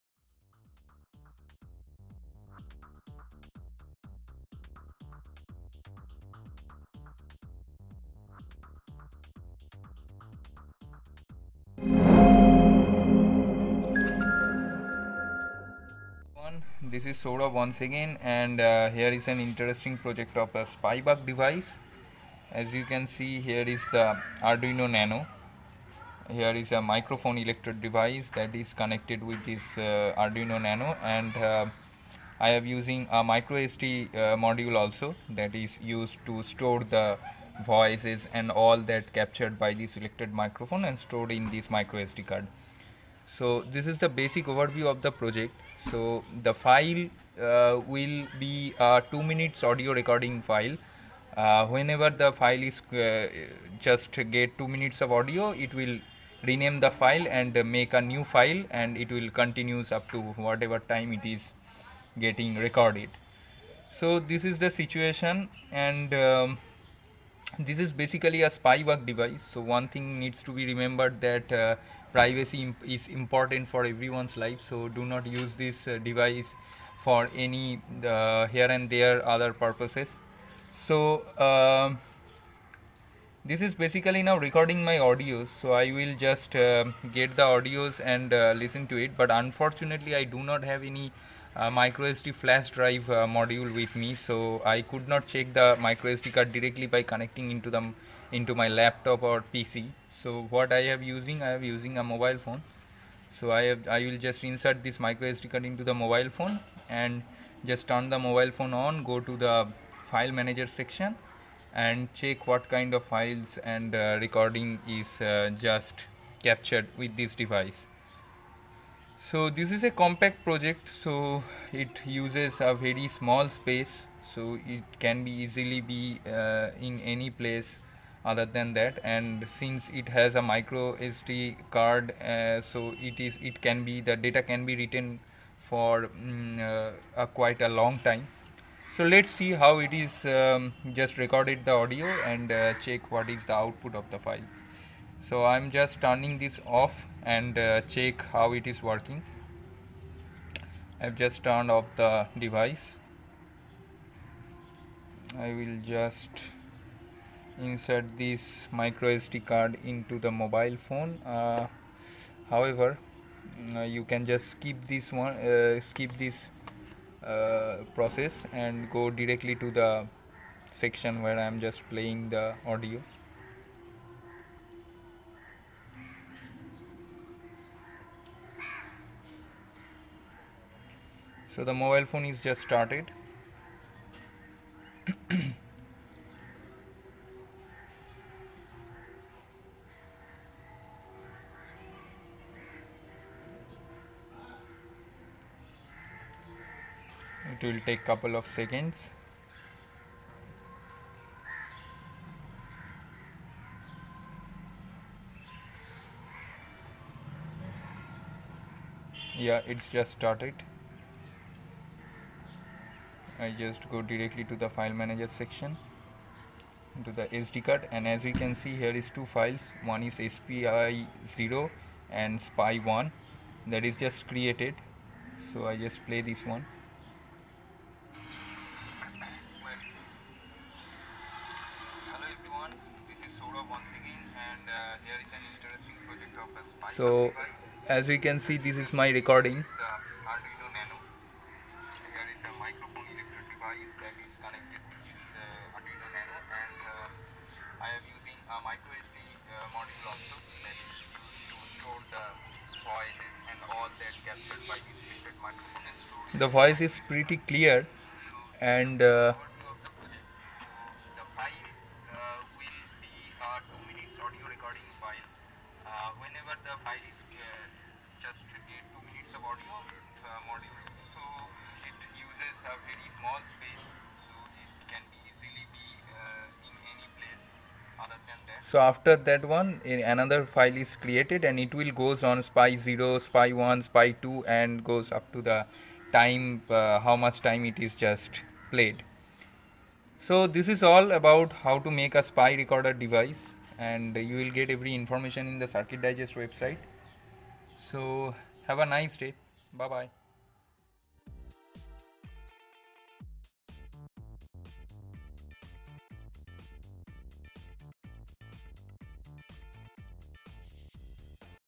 How to Make Spy Recorder Device using Arduino | Arduino Voice Recorder | Arduino SPY Bug
Here we decided to build our own Arduino Based Voice Recorder, which can also be used as a spy bug. Our spy bug uses a small microphone to record the voice and it stores the recorded voice into an SD card. It will able to record audio clips that are 2 minutes long, each 2 minutes long clip would be numbered serially. Once powered up the recording process will continue until the battery is dead or there is no more space to store the recorded audio. We also attached an LED with the Arduino that indicates the recording process is started.

Warning: The project discussed here is only intended for educational use. Respect other people’s privacy, any misuse of this device might be a punishable offense according to your state law.